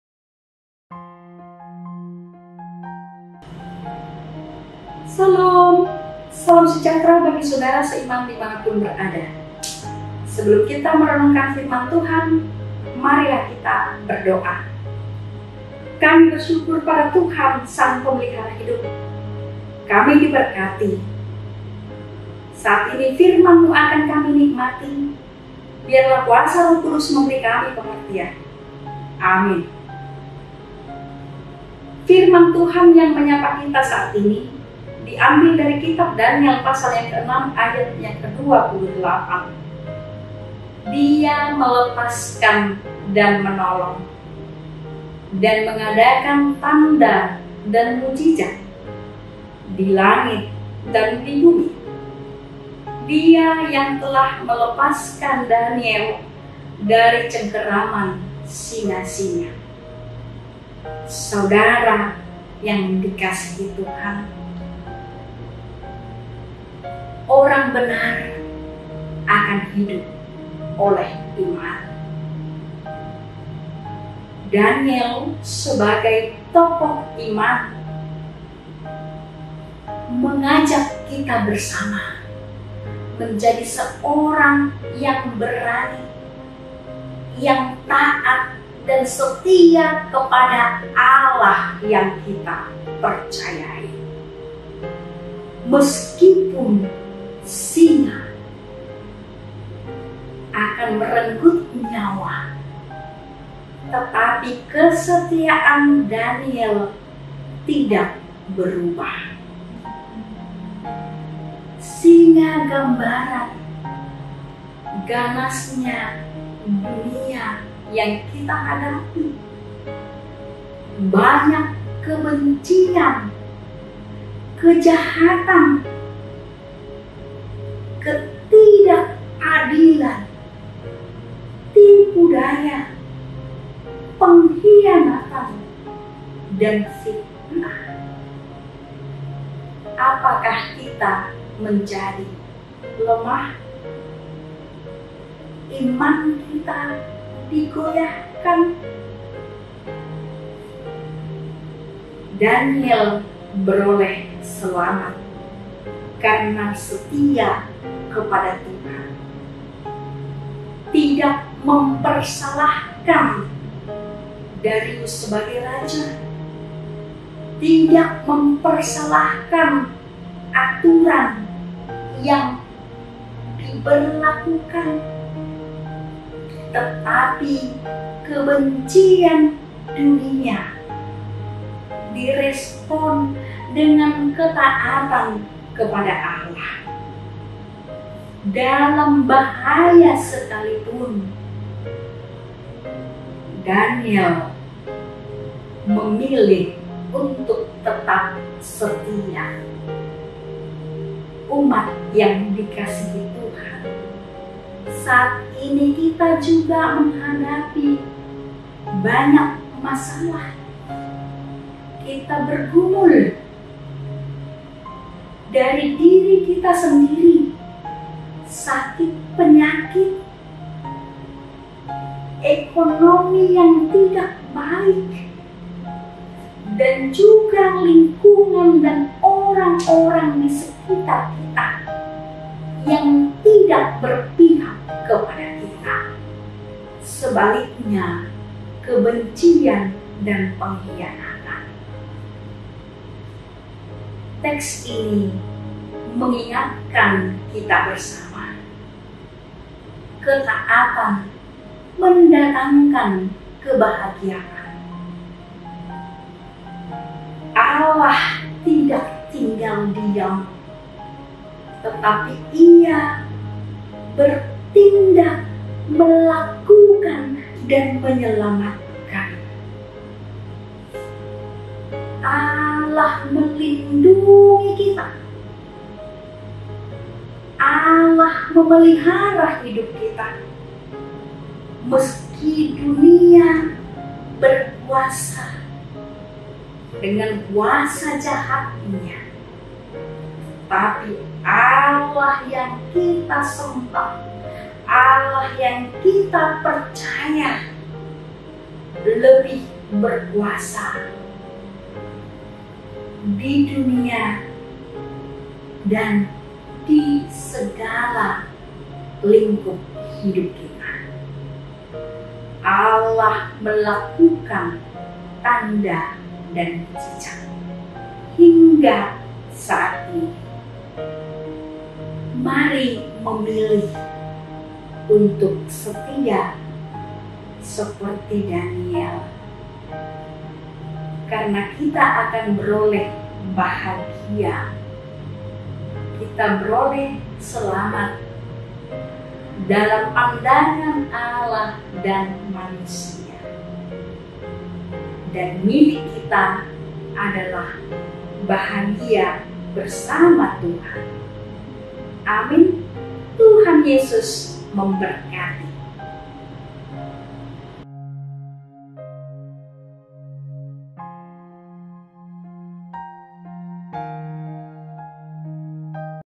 Renungan (Daniel 6 : 28)|Pendeta Suryani Siregar, S.Th, MM|"Mukjizat masih Ada"
Renungan
Pendeta Suryani Siregar, S.Th, MM

"Mukjizat masih Ada"

Dia melepaskan dan menolong, dan mengadakan tanda dan mujizat di langit dan di bumi, Dia yang telah melepaskan Daniel dari cengkaman singa-singa.”

Saat kita mengalami masalah, ada rasa kuatir cemas dan takut, Daniel memberi teladan untuk tetap taat dan tidak mempersalahkan yang lain. Mari tetap tenang walau tegang, Tuhan adalah sumber Keselamatan kita. GBU🙏🙏